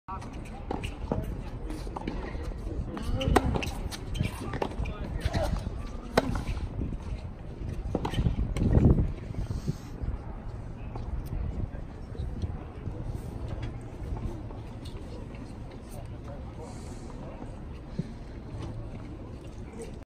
Marin Cilic Practice
Indian Wells BNP Paribas 2019. ATP WTA Tournament.